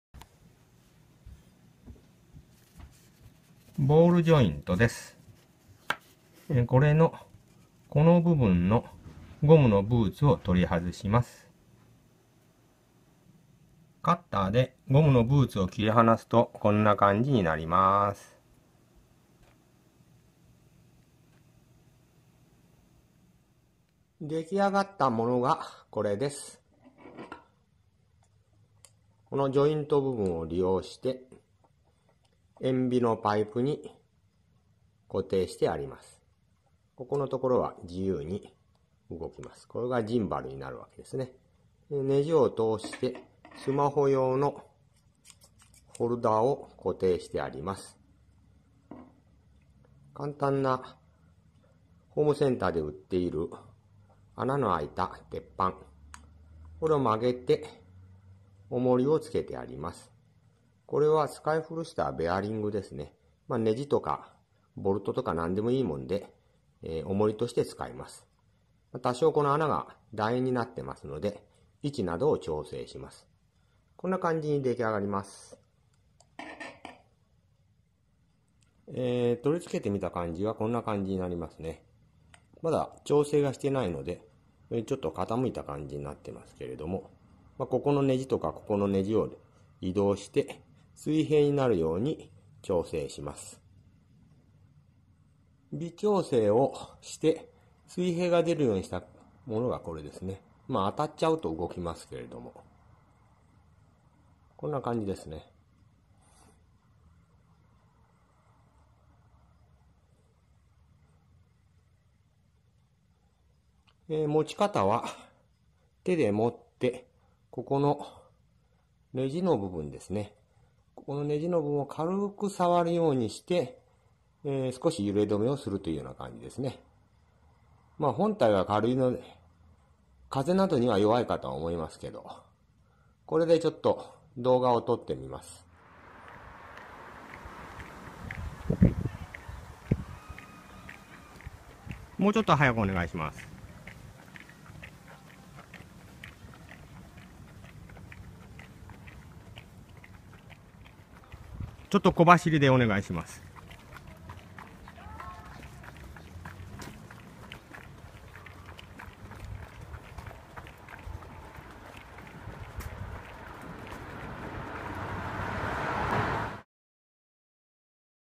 格安自作～iphoneやスマホ用カメラスタビライザー(片手持ち用)を材料費1000円ちょっとで作ってみた
工業ボールジョイント（Ball joint）をジンバルとして利用した、簡単＆安価に作成できる、スマホ用のStabilizerの紹介です。

両手を使わずに、片手だけで使えるタイプです。 You can use with one hand retention.いわゆるアイスクリーム型です。

Creating article of Handheld Video Steady Cam .
費用は、1000～1500円くらいです。サイズ的には最小クラスになります。

ヤジロベー式（非電子式）のスタビライザーは、カメラ部が軽量になればなるほど、安定性がなくなる物理特性がありますので、大きな効果は期待できませんが、それでも十分な効果はあります。

動画の最後に比較動画をつけてありますので、見てください。

スマホホルダーのネジは、インチネジですので、ボールジョイントには無理やりねじ込んであります。

ヌルヌル感を出すには、スマホ側と下部に重りを追加して全体の重量を上げる必要がありそうです。

すこし手を加えれば、Goproなどにも使えます。

小型で軽量のカメラ向きですが、重りを増やせば大型カメラ用にも出来そうです

※ジンバル部分のボールジョイントが小さいので、全体が大型になればなるほど、てこの原理が効き、動きがスムーズになります。

小型のカメラスタビライザーとしては、MOTES SK-W02 、steadycam GH2 、ModoSteady 585、 Manfrotto stabilizer 、 DCS236-RD、 ifootage wildcat2、 SPEED MINI 、WildCat2、など沢山の製品がありますが、高価なので今回のように自作も良いと思います。